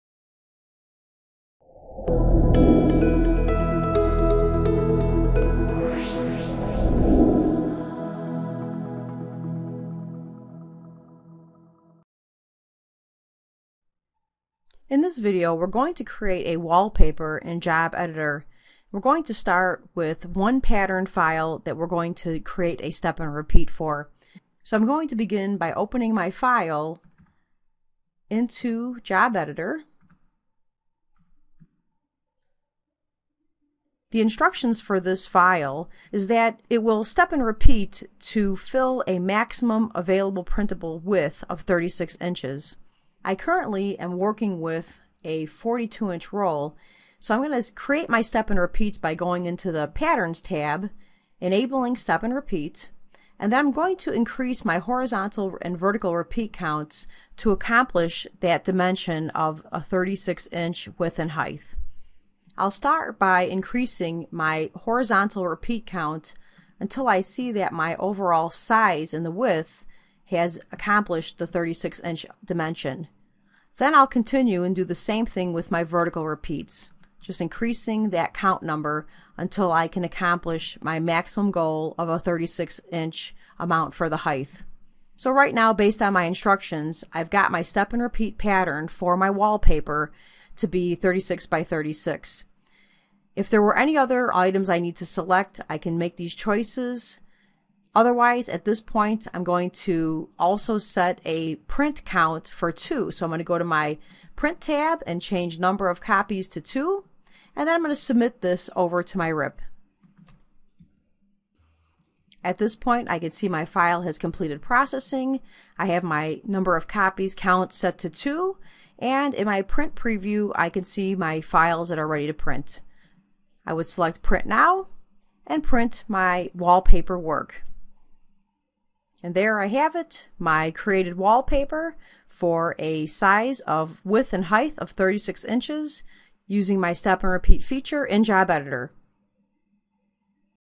ONYX Software: How to create wallpaper
In this training video, we are going to create a wallpaper in Job Editor. Our ONYX expert starts with one pattern and uses the flexible step-and-repeat process.